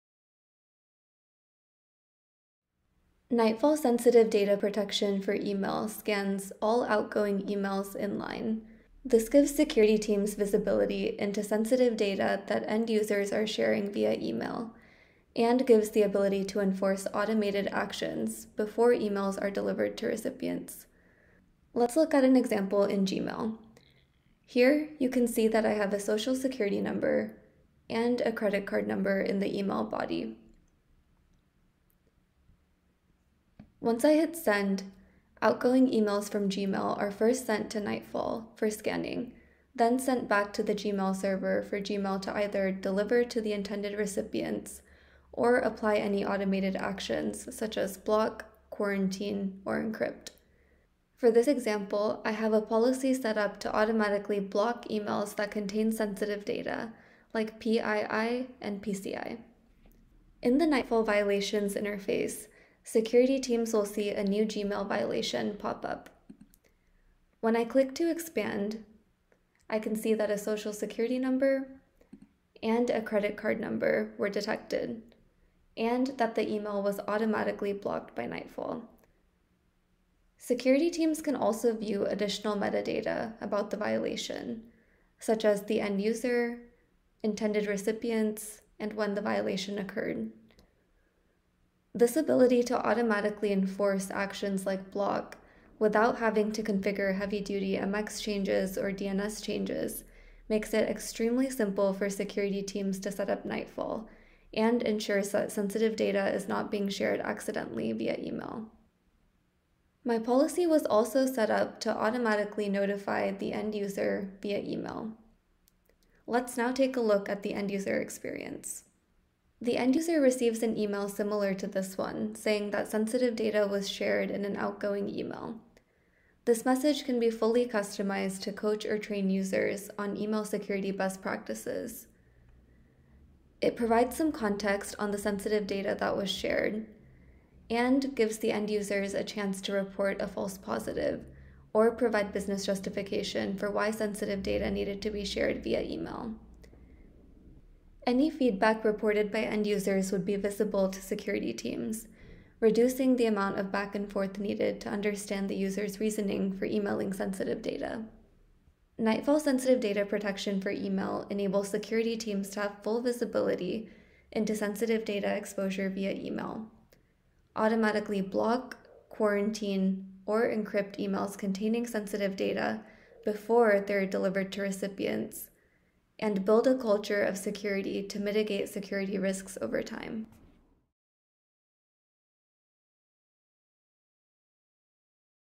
Nightfall Sensitive Data Protection for Email
Leverage Nightfall’s AI-native platform to pinpoint and protect PII, PCI, PHI, secrets, and credentials across SaaS and email, including Gmail.

Built with AI at the core, Nightfall Sensitive Data Protection is transforming email DLP by helping security teams to…

… detect sensitive data with 2x better precision and 4x fewer false positive alerts.

… act swiftly by blocking or quarantining emails, or removing attachments that contain sensitive data.

… protect granular instances of sensitive data via encryption, redaction, deletion, or quarantine.

… educate end users with real-time notifications about company policies.

Want to see Nightfall in action?